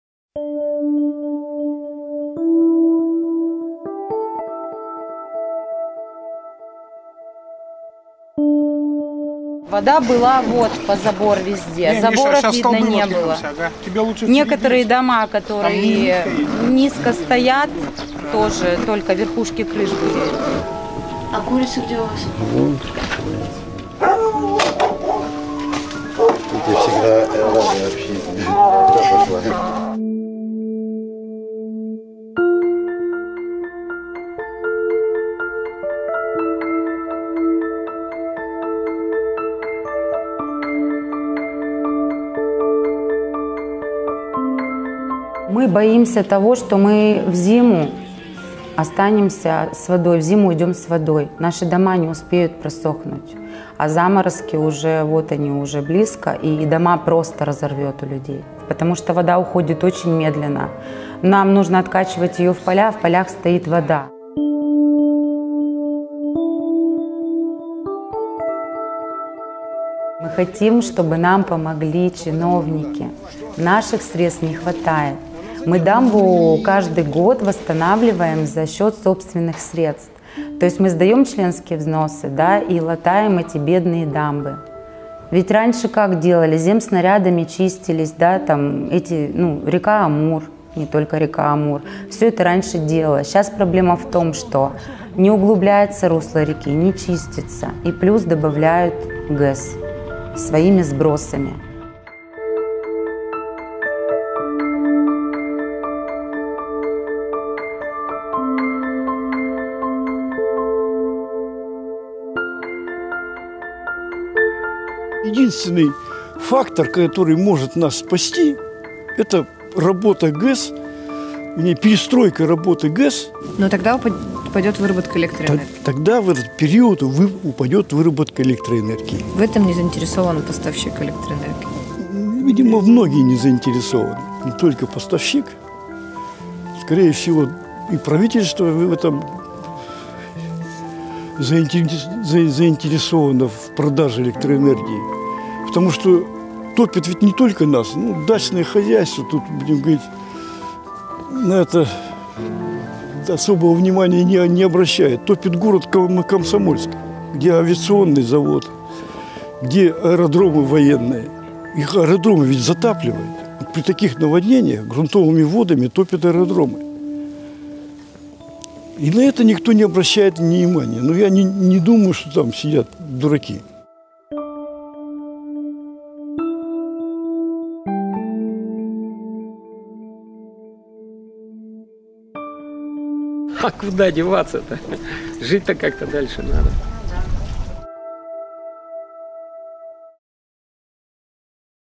Дачники на митингах в поддержку Фургала в Хабаровске | Сибирь.Реалии
На митингах в поддержку Фургала выходили не только его сторонники, но и люди, которые не нашли иного способа достучаться до власти. Например, хозяева дач недалеко от Хабаровска, их затопило еще в сентябре при разливе Амура. Несколько месяцев из их домов не уходила вода, но чиновники и МЧС не оказали помощи